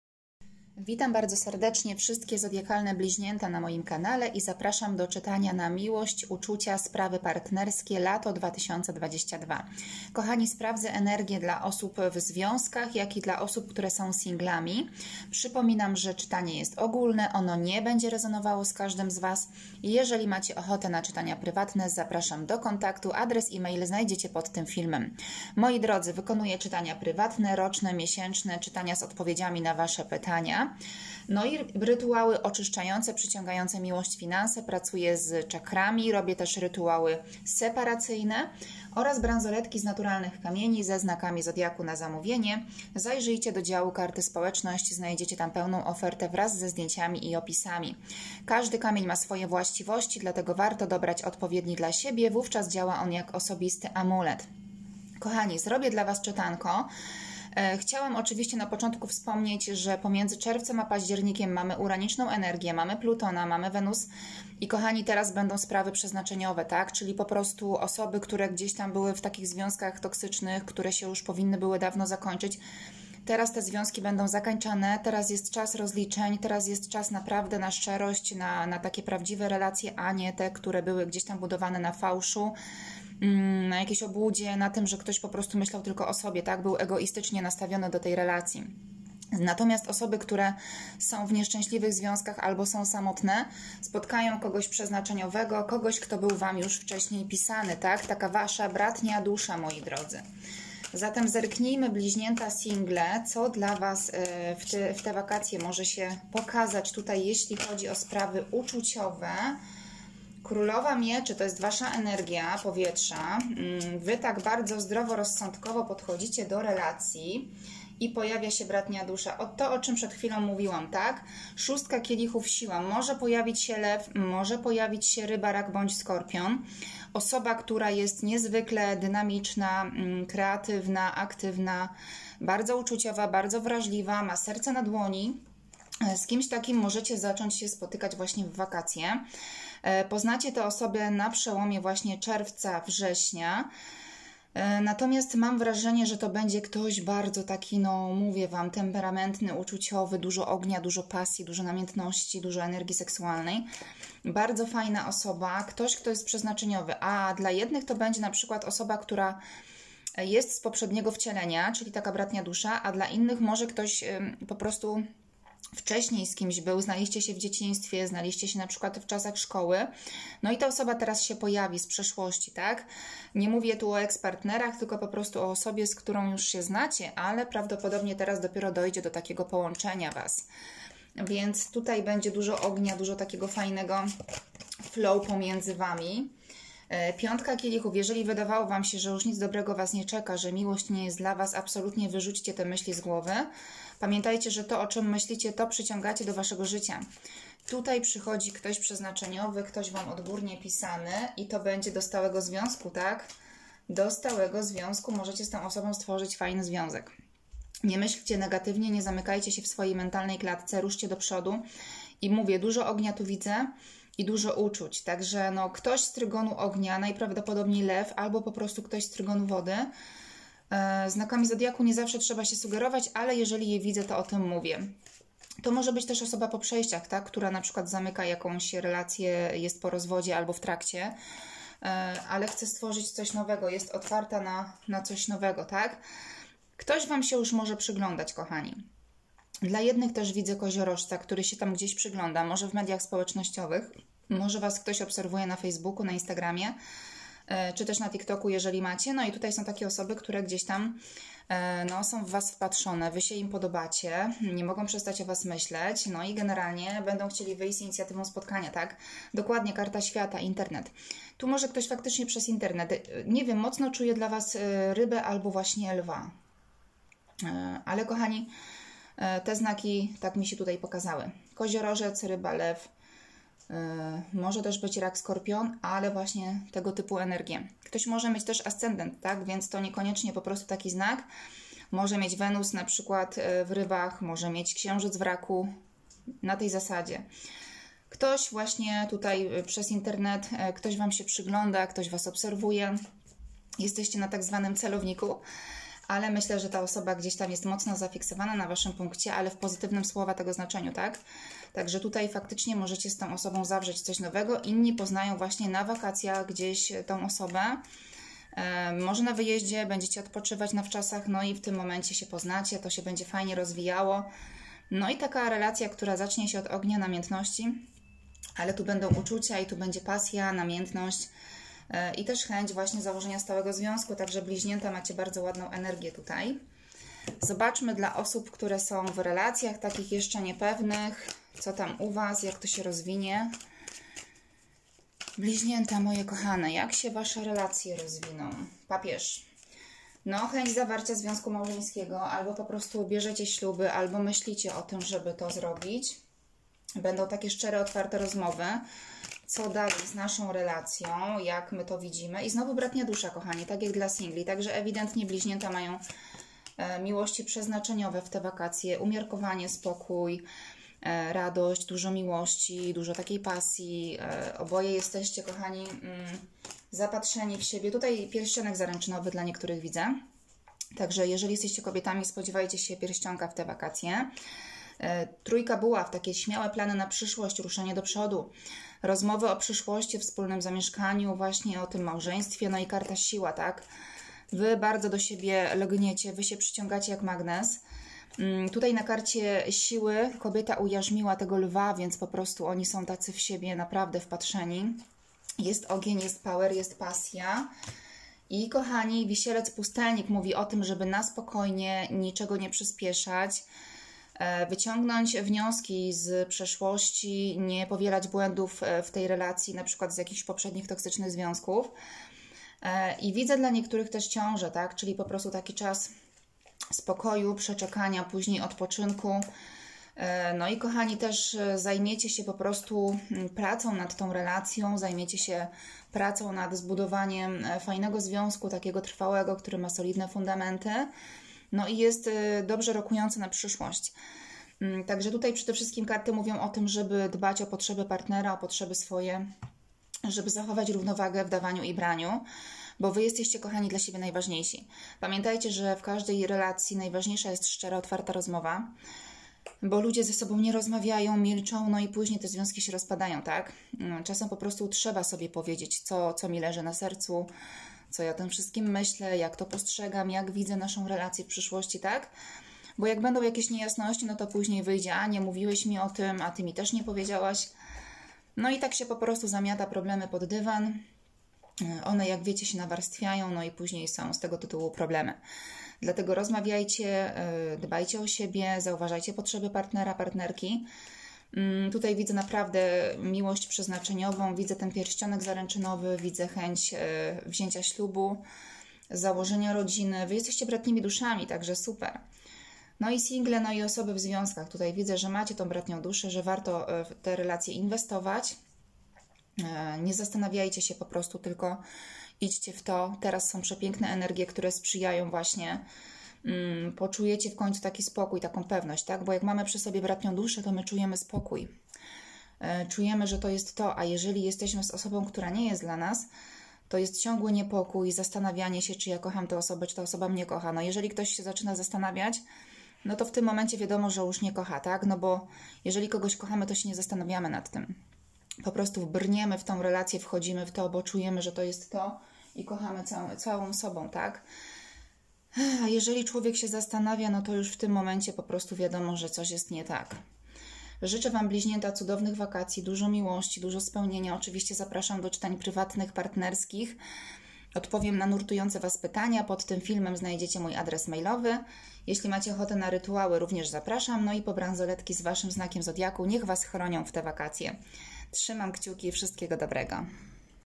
♊BLIŹNIĘTA ♊❤️miłość ❤️ lato 2022 / bratnia dusza z przeznaczenia 😍🙏
Zapraszam do czytania na miłość lato 2022.
Pamiętajcie że jest to czytanie ogólne i nie z każdym będzie rezonować.